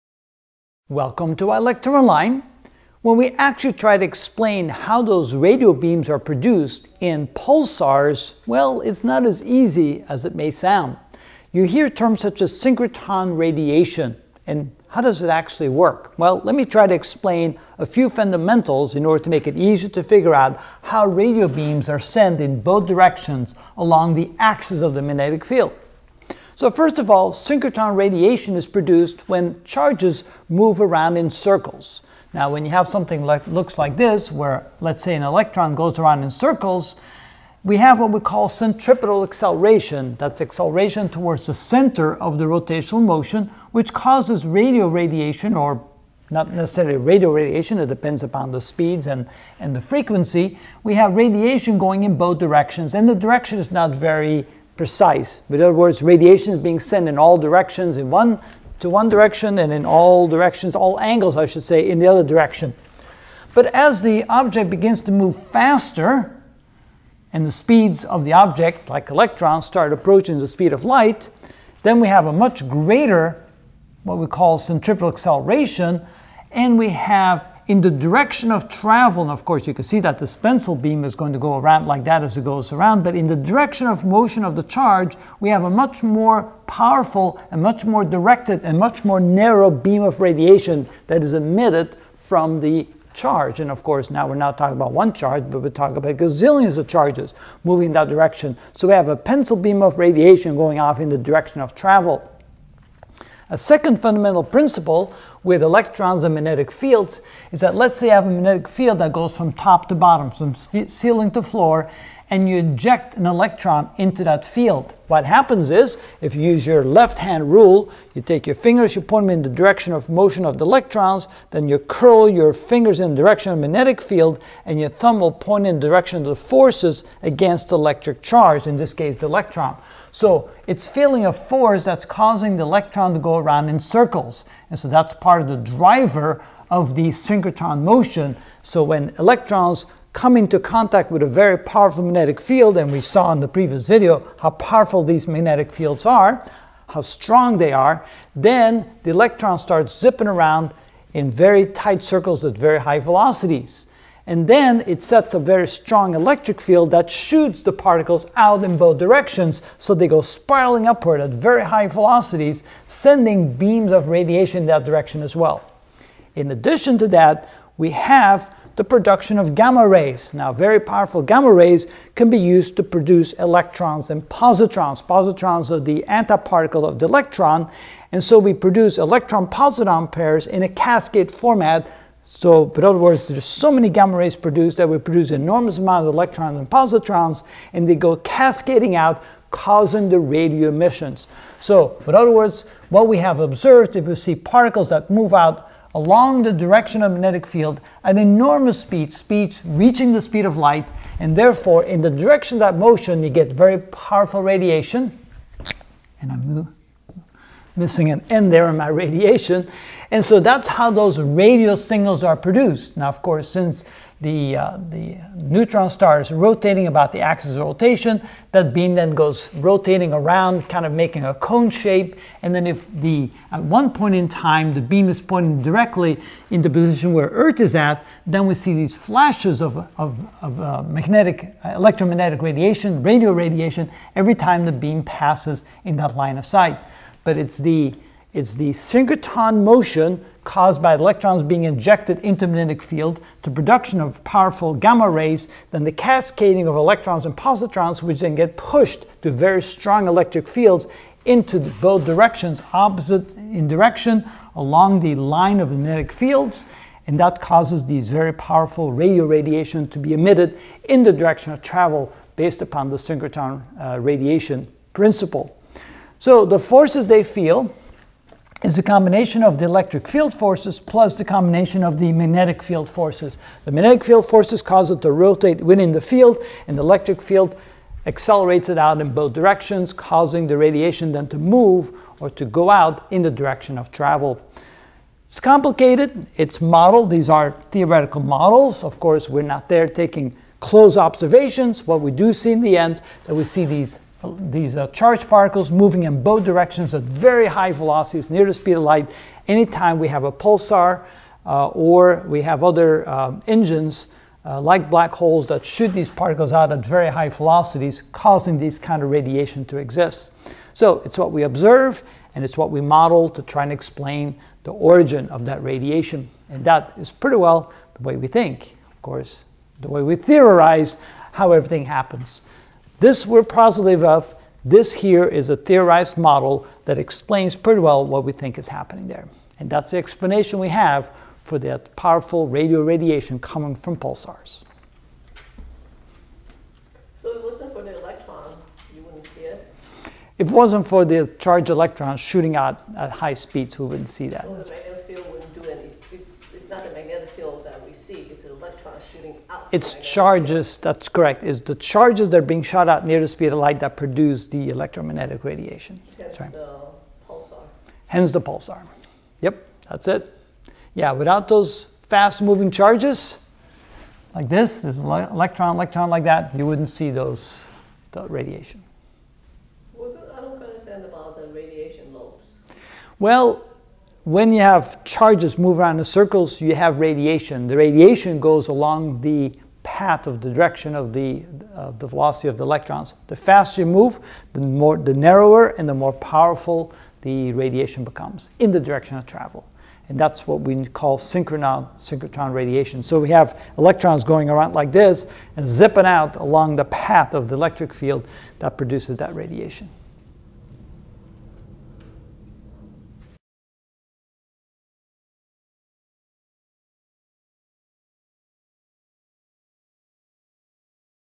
Astronomy - Ch. 22: Neutron Star (14 of 17) Synchroton Radiation in Pulsar [ F = qE + q VxB]
for more math and science lectures!  Note the error in the force equation: remove the "c"

We will learn what is the synchrotron radiation in the radio beams in pulsars.